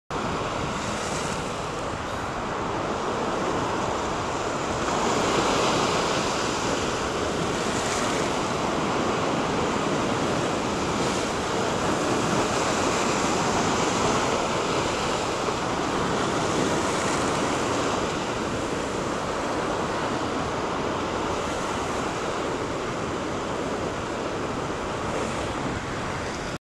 Natuur in Zuid Holland: Kanoeten op de vlucht voor de golven
Kanoeten op de vlucht voor de golven op de Brouwersdam.

Vogels kijken op de Brouwersdam
Wist je dat je op mijn website meer kunt lezen over vogels kijken op de Brouwersdam? Ga naar